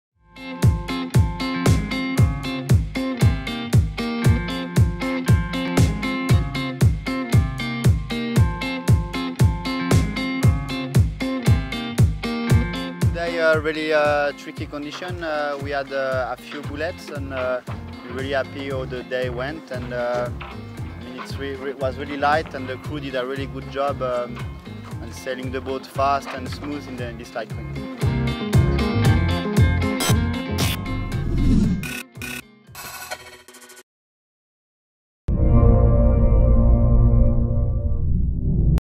Smooth flying on Marseille One Design
Erik Marik’s Zoulou scored a perfect scoreline on day three of Marseille One Design. The French team has pulled out a significant lead of 11 points going into the final day of racing tomorrow.